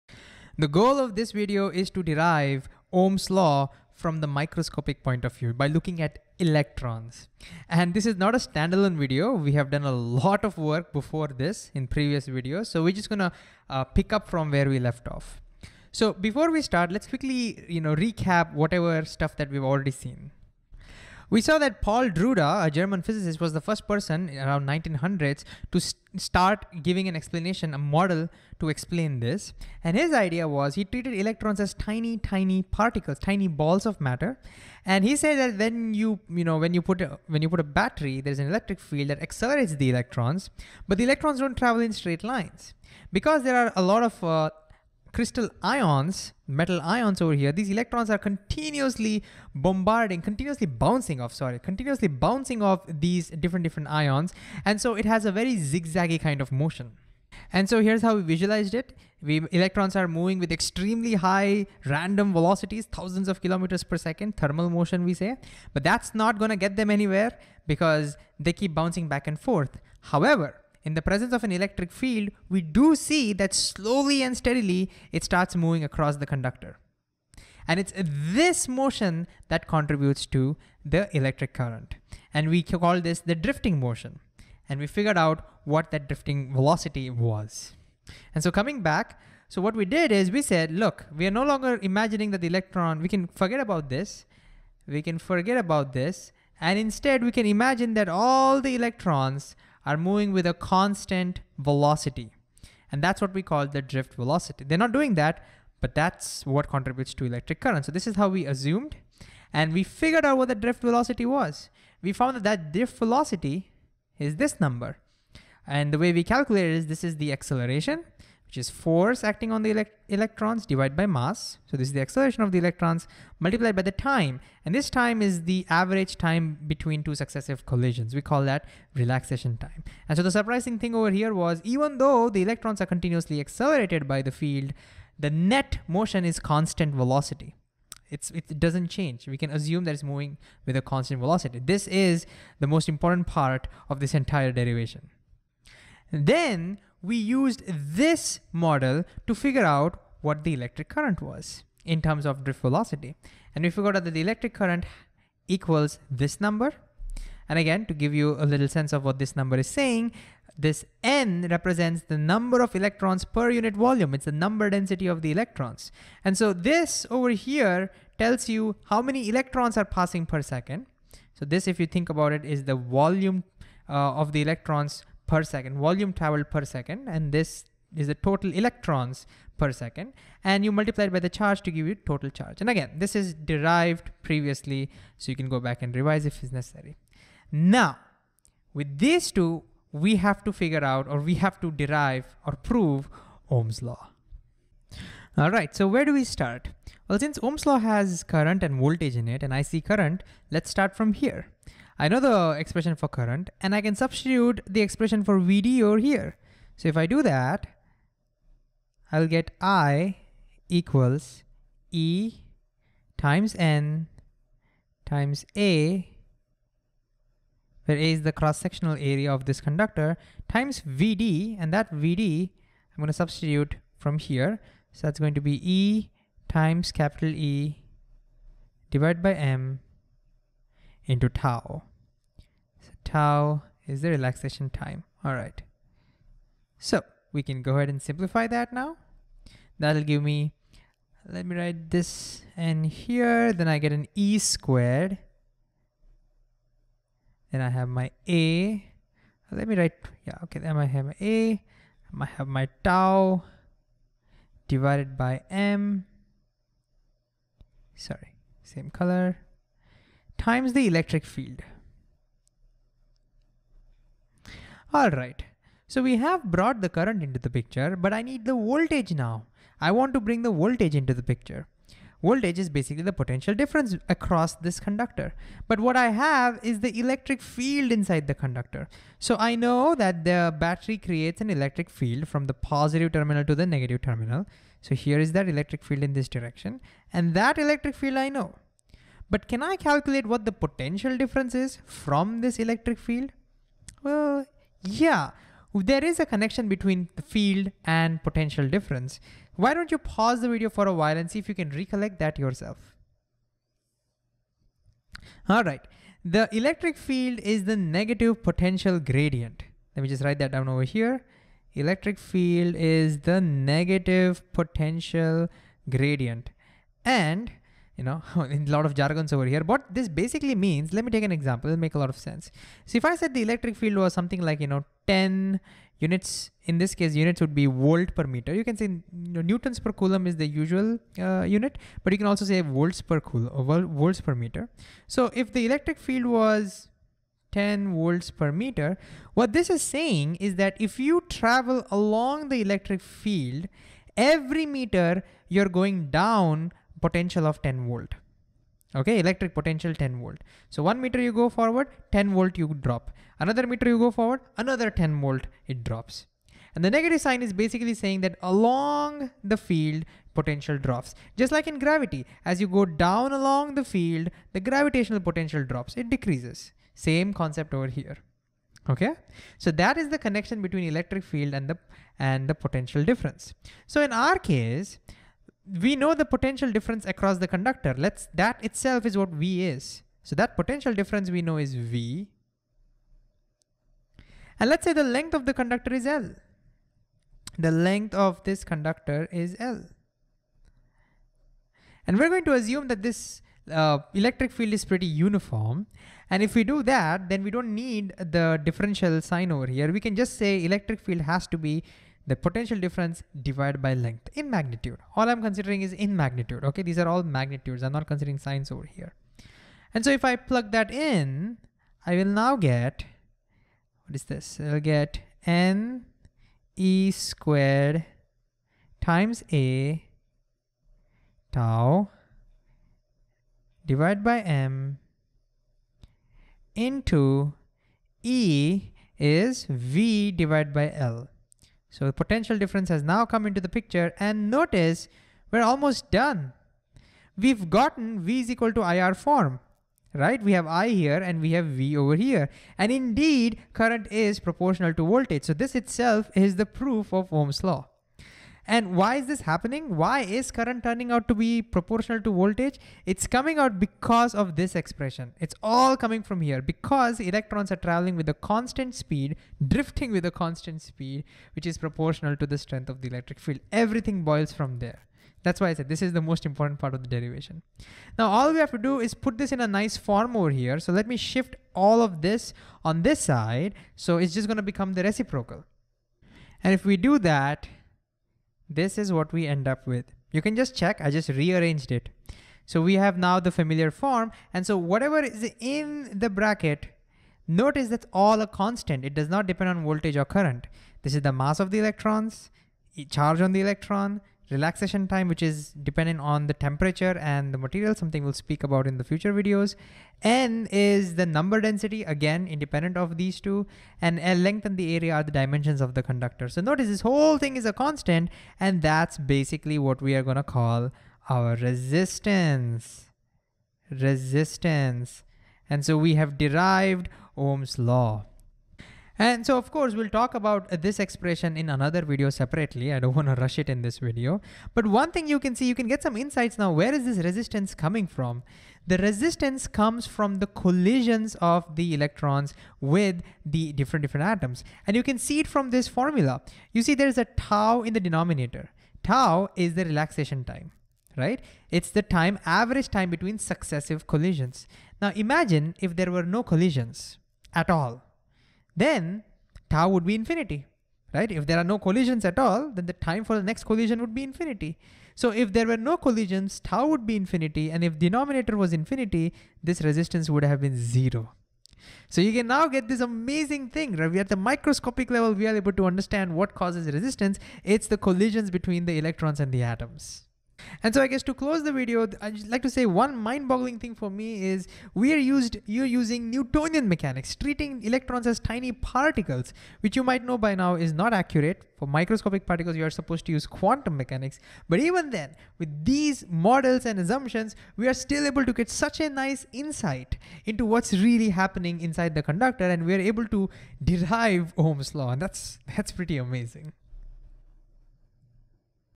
Ohm's law - derivation (using drift velocity)| Electricity | Physics | Khan Academy
By using the expression for drift velocity, electric current (in terms of drift velocity), and connection between electric field and potential, we can derive Ohm's law.